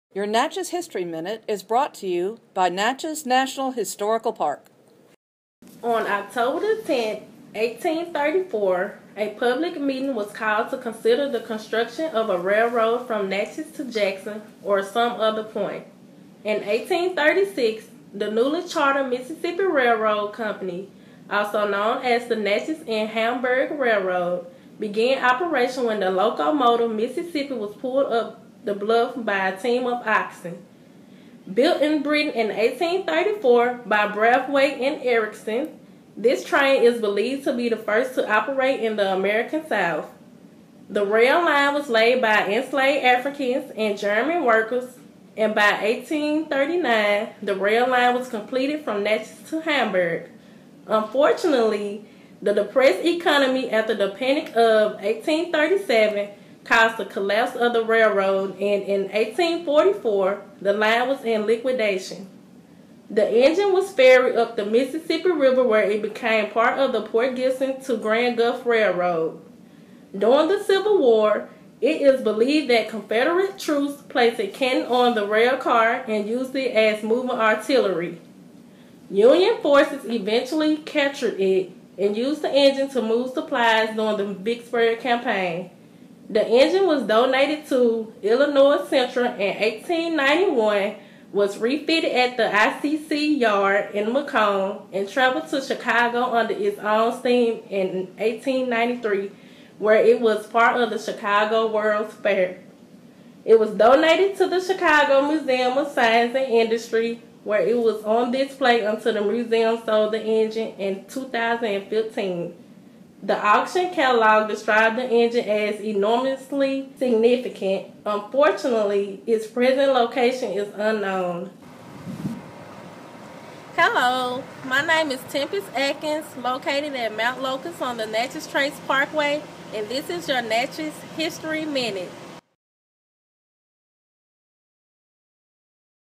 Oct 9 A Railroad for Natchez
Tempest Atkins presents today's Natchez History Minute about a public meeting held in October,1834 to consider the creation of a railroad. The Natchez to Hamburg railroad began operations once the locomotive "Mississippi" was hauled up the bluff. Manufactured in Britain, the "Mississippi" is believed to have been the first locomotive operating in the South and until recently, was on display in Chicago.